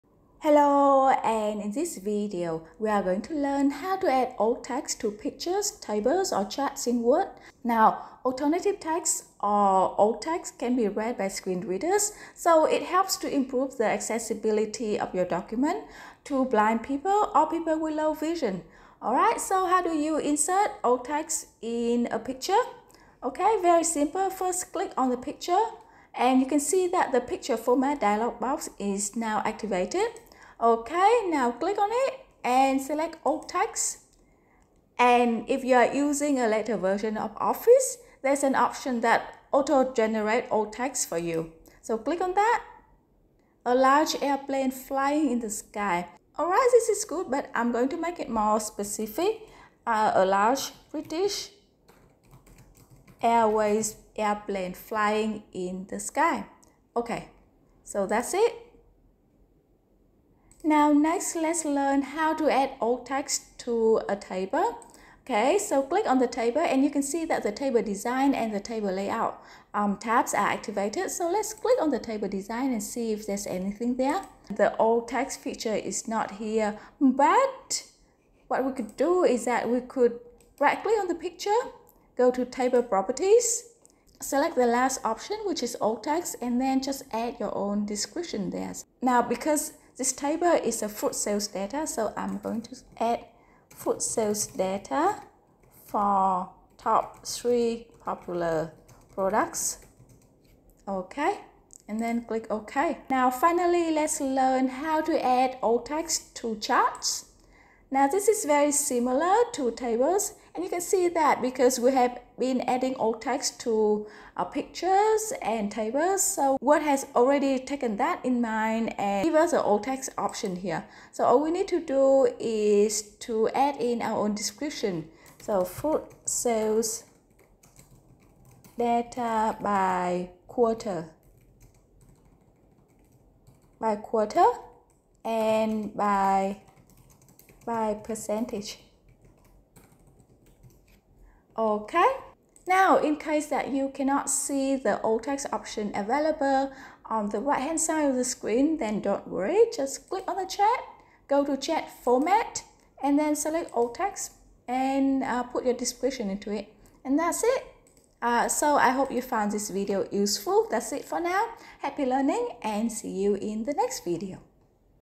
How to add alt text to images, tables and charts in a Word document
Alternative text or alt text helps improve the accessibility of your document to visually impaired readers, or those with low vision.  Let's learn how to add alt text easily and effortlessly to different types of objects, such as pictures, tables and charts in Word, all in in this 3-minute video.  Let's go!!!